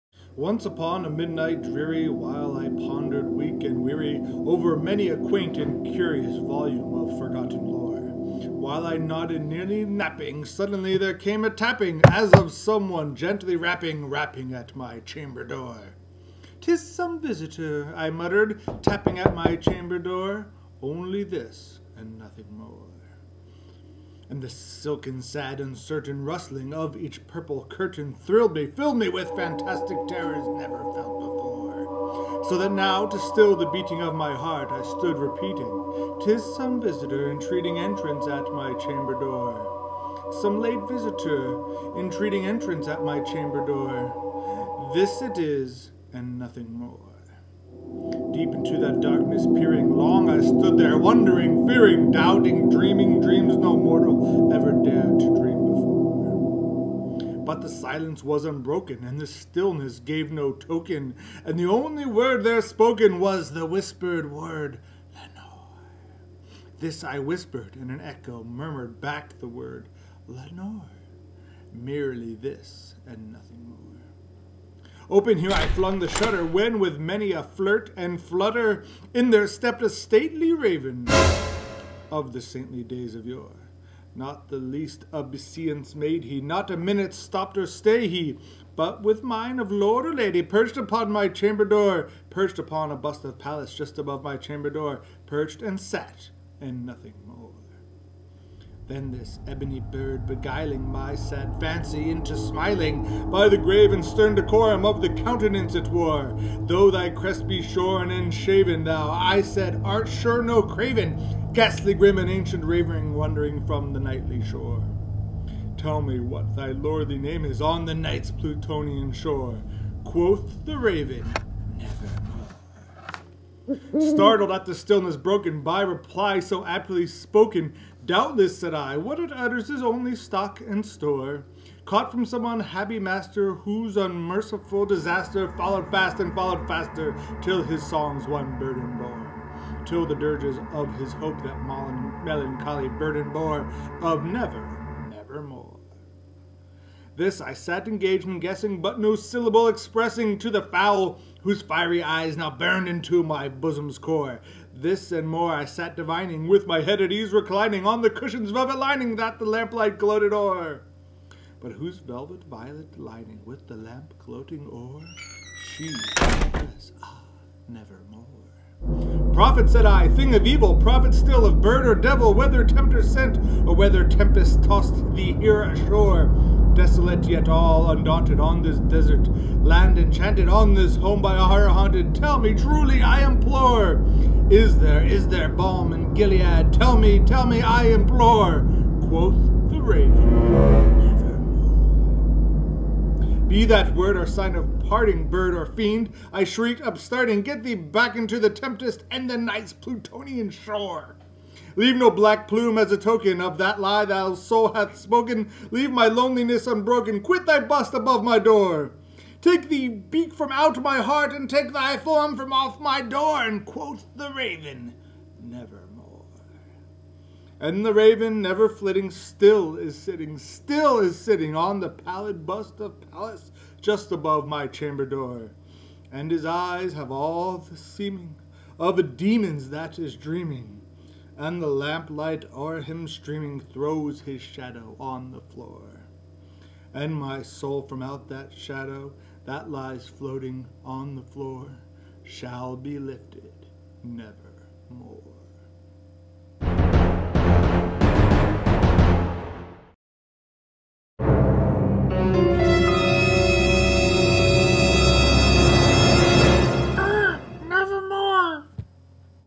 "The Raven" - Edgar Allan Poe (read by Josh Berk of the Bethlehem Area Public Library)
BAPL Director Berk does his best Poe.